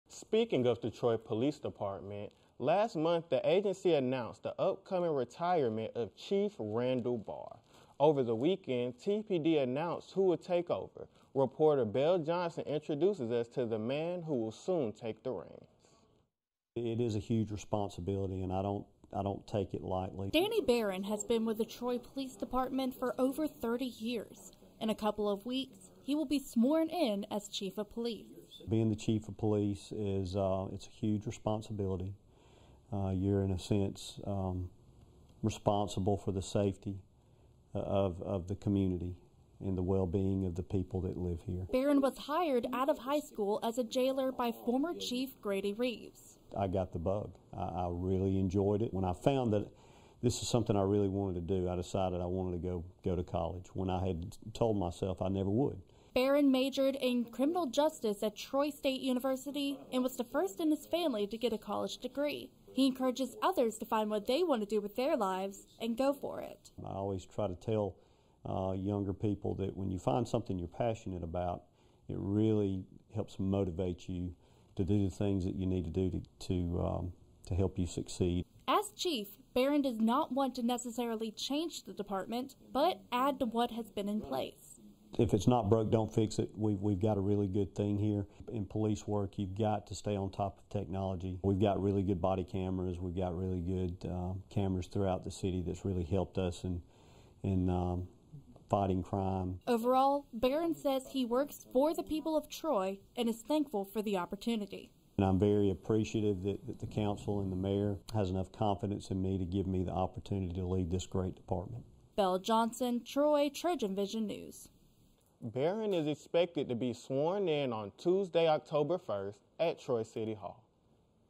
New City of Troy Chief of Police - TROY TrojanVision News
Speaking of the Troy police department, last month the agency announced the upcoming retirement of chief Randall Barr. Over the weekend, TPD announced who would take over. Reporter Belle Johnson introduces us to the man soon taking the reigns.